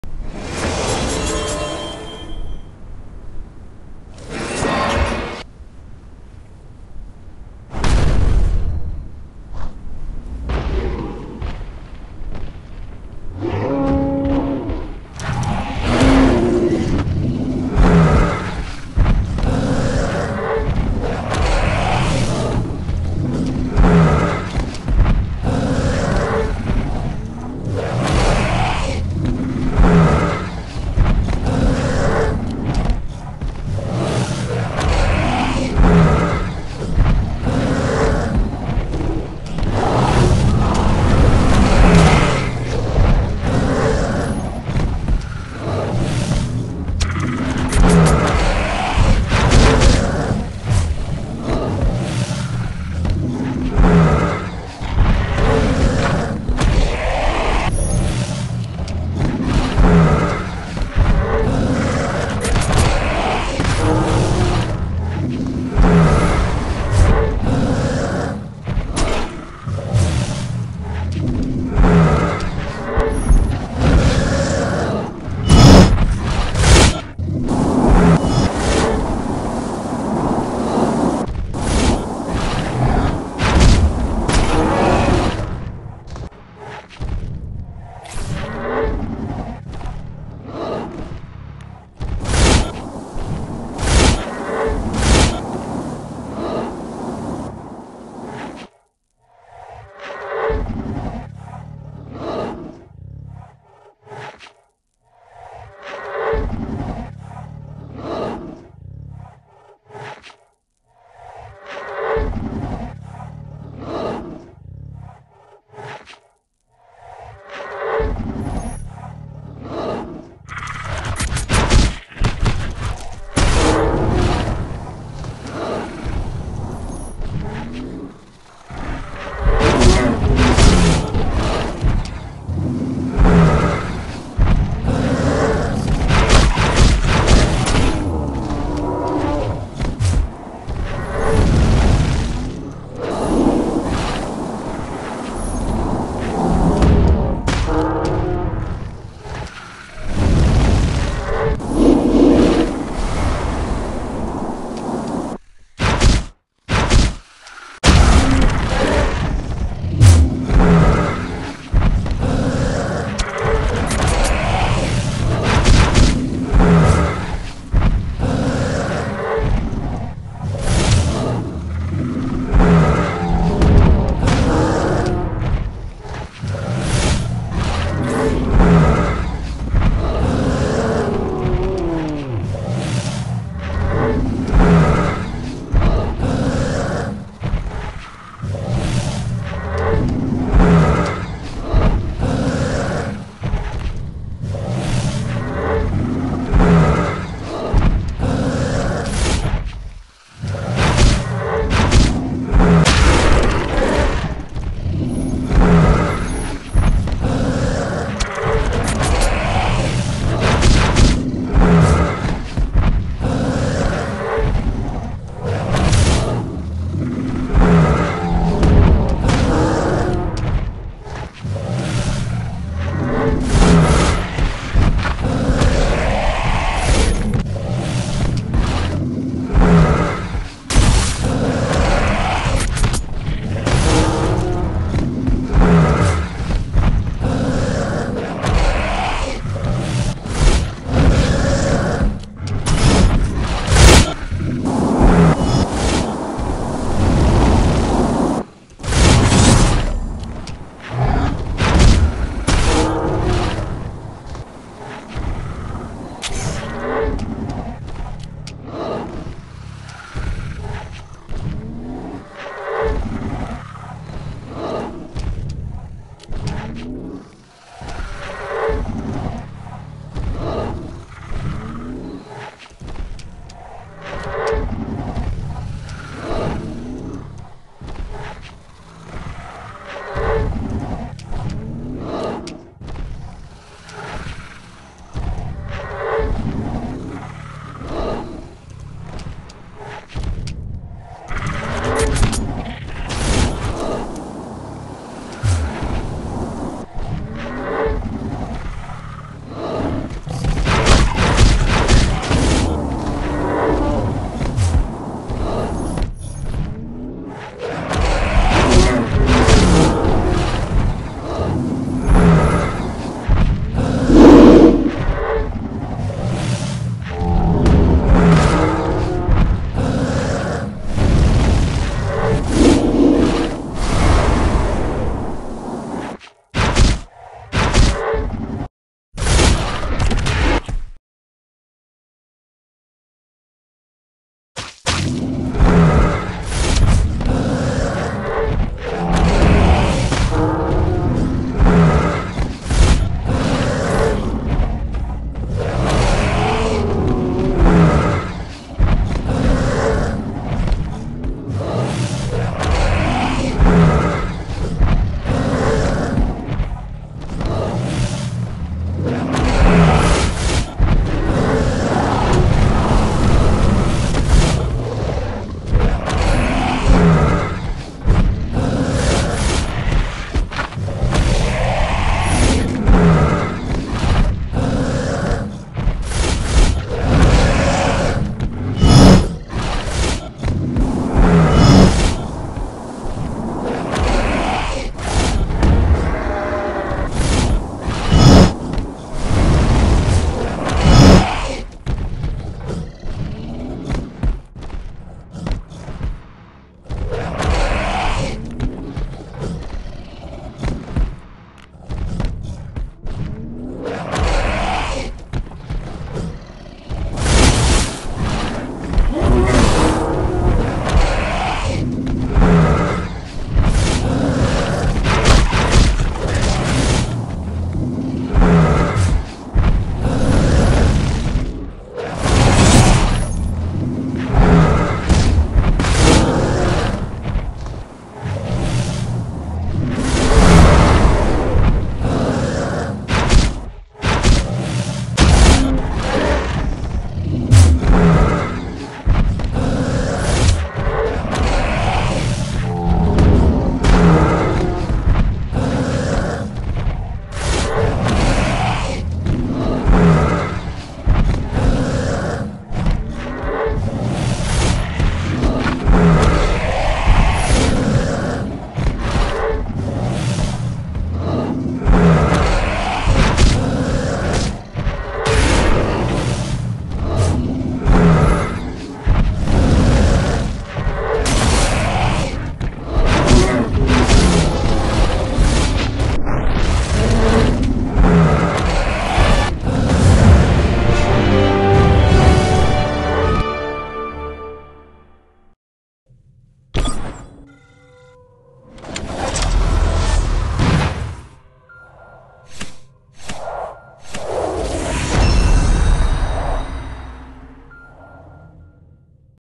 My Team vs The Epic Brachiosaurus: Boss Raid. (No Background Music.) (2/2.)
I came back into this raid once again to defeat the Epic Brachiosaurus, but in a new team, even though I brought in a group shielder, well, make that 2 group Sheilders, I thought we weren’t going to be able to defeat the Brachiosaurus, but in the end we did it, we’ve managed to defeat the Brachiosaurus, what do you think about the Brachiosaurus’s Appearance, was it awesome or was it interesting?.
I love to hear what you’re thinking in the comment section below.￼